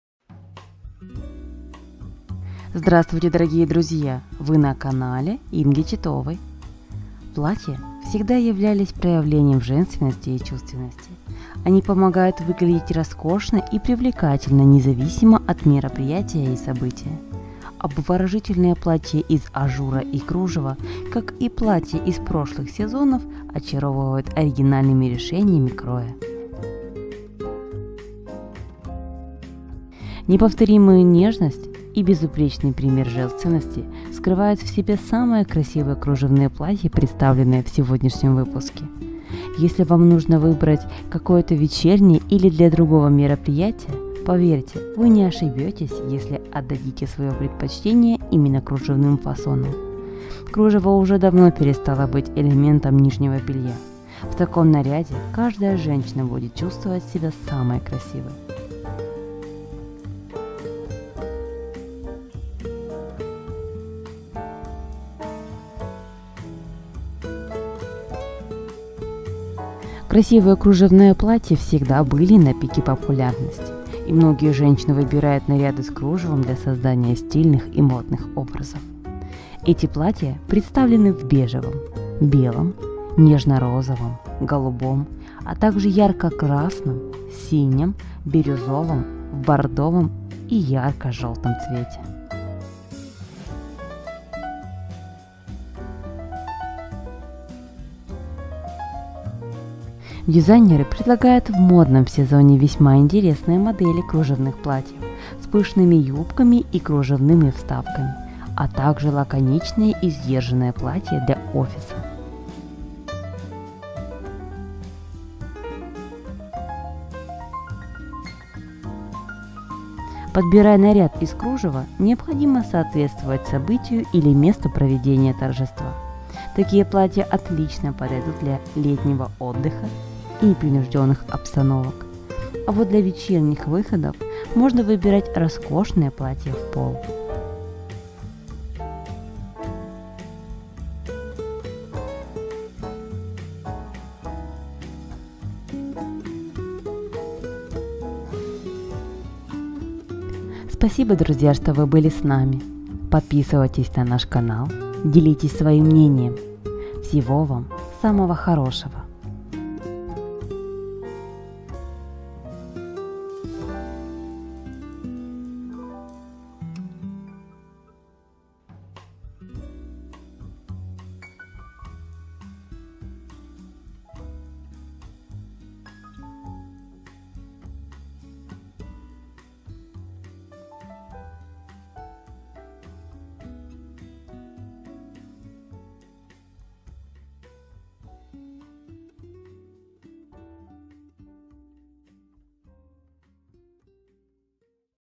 КРАСИВЫЕ КРУЖЕВНЫЕ ПЛАТЬЯ
Самые красивые кружевные платья – это всегда восхитительные и женственные образы, позволяющие выглядеть роскошно и привлекательно независимо от мероприятия и события.
Моя жизнь, творчество, вдохновение.
Дорогие и прекрасные мои зрители, буду очень рада дружить и общаться с вами!